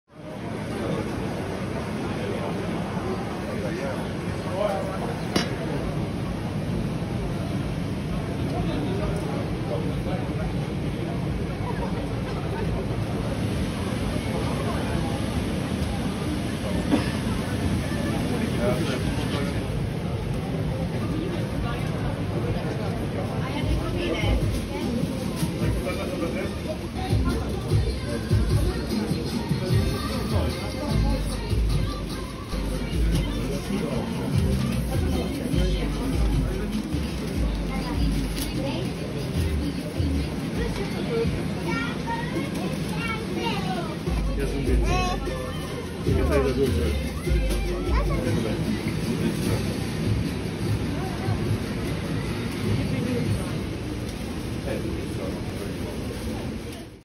Candy Shop Budapest
Amazing pastry shop with a huge range in the center of Budapest. Fun for you and your children is guaranteed!